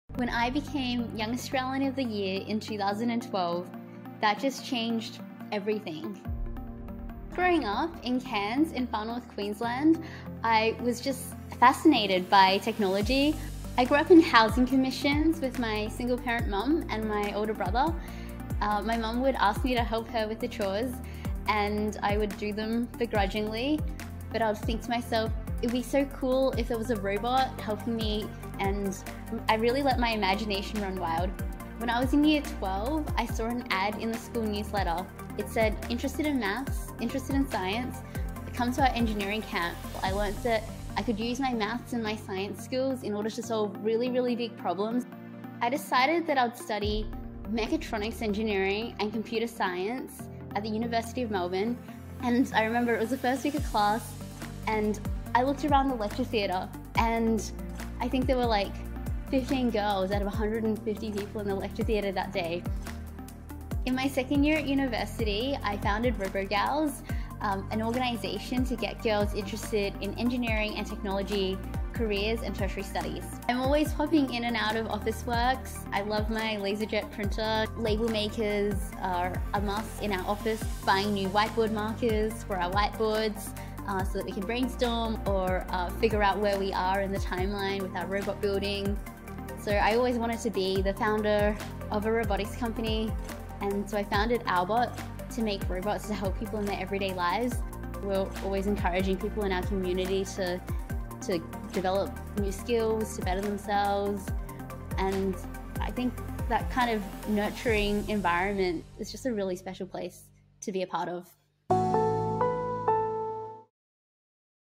In the Beginning: Marita Cheng
Growing up in Cairns, Robogals founder Marita Cheng imagined a world where robots helped mankind. Since then, she’s been making waves as a mechatronics engineer.

Watch new videos each week, including great craft projects to do with your kids, reviews of the latest tech and expert business advice: @Officeworks